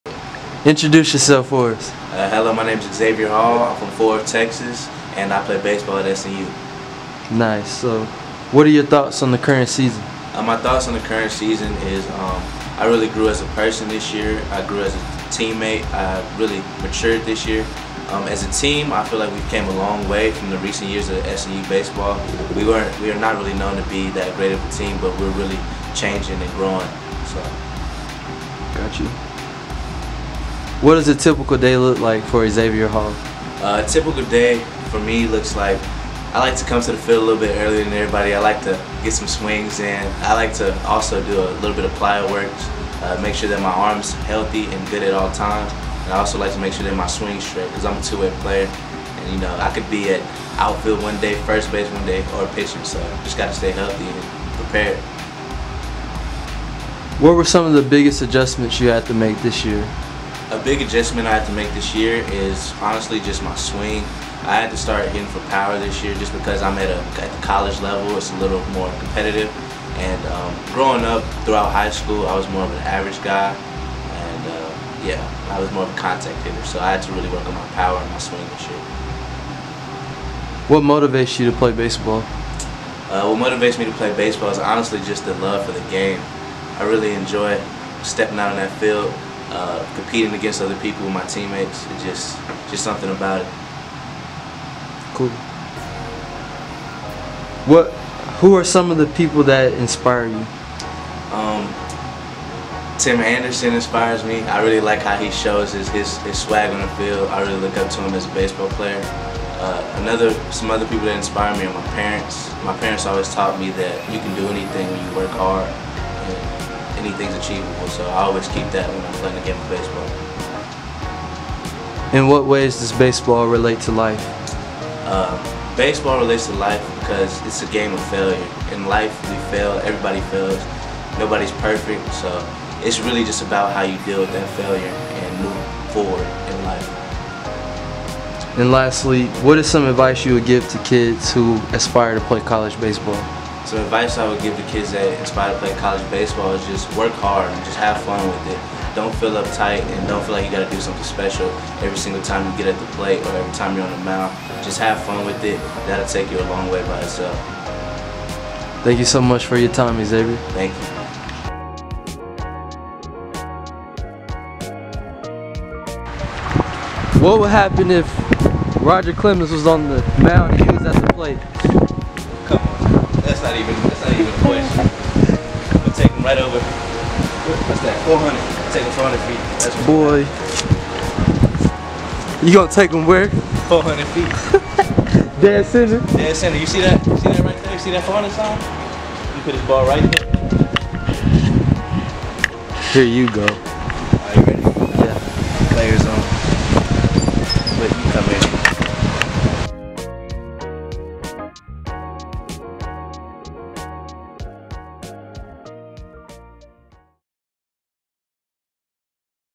One on One with Xavier Hall (Freshman P/OF/1B, Southern Nazarene University)
Interview with Freshman student athlete Xavier Hall